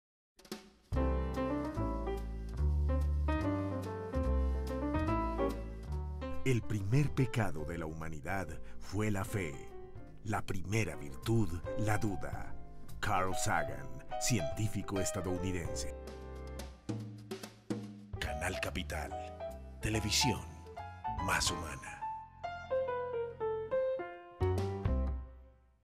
Personaje Carl Sagan
"El primer pecado de la humanidad fue la Fe; la primera virtud fue la duda": Carl Sagan, científico estadounidense, personaje de la TV más Humana.